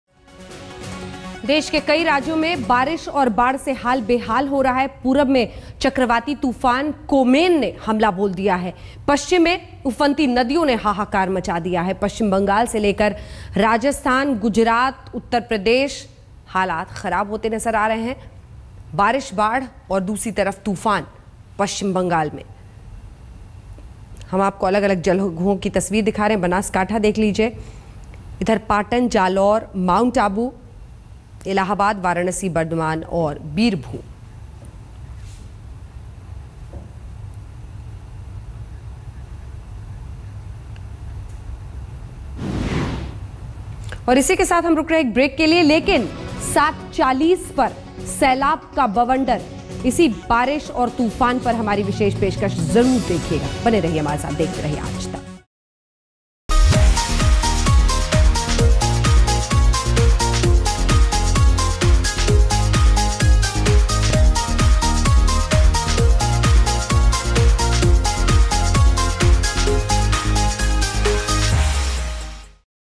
India 360: Rain And Floods Cause Havoc In India
Ceaseless monsoon rains have caused widespread damage to public and private properties in most parts of India. Due to continual rains lashing parts of Gujarat, small and large dams and reservoirs in the state have filled to the capacity. Listen in.